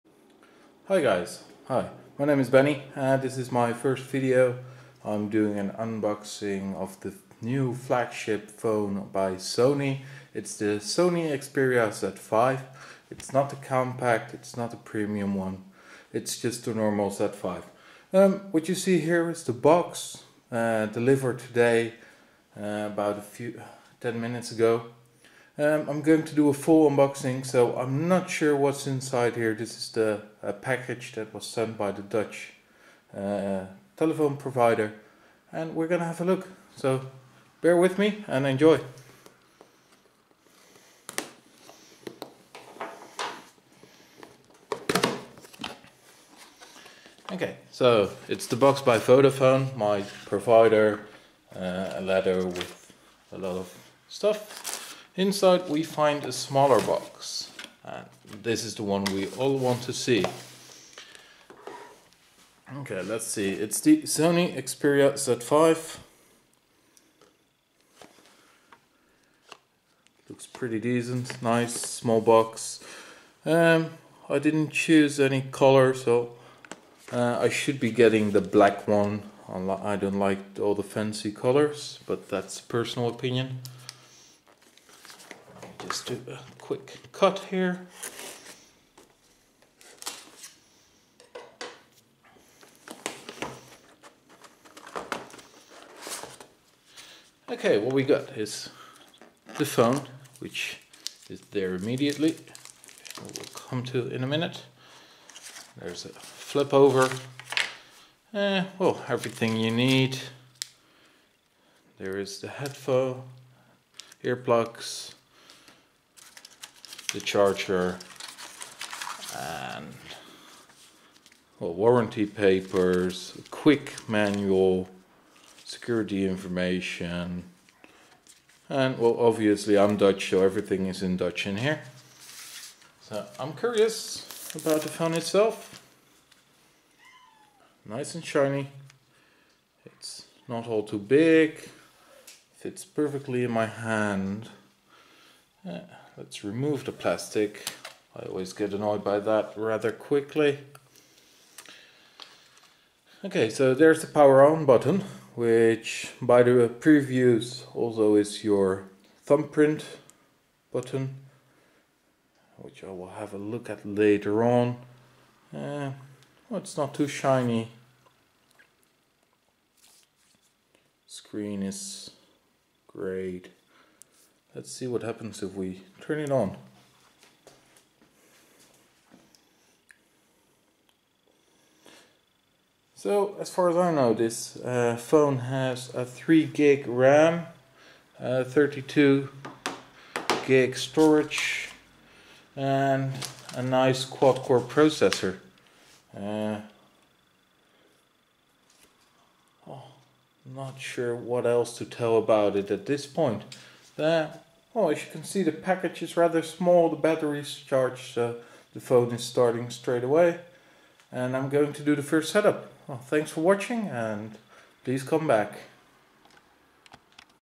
Sony Xperia Z5 Unboxing First startup HD
This Video shows the unboxing of the new 2015 Flagship of Sony.
You will see me open all boxes from the start and giving me a little discription of what I see.

It is a one shot upload, you can only do an un boxing once!